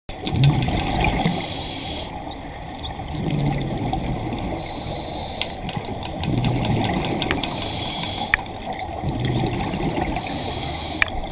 Lovely red sea dolphins n best selfie underwater .
Dive with Red sea dolphins as scuba buddy ..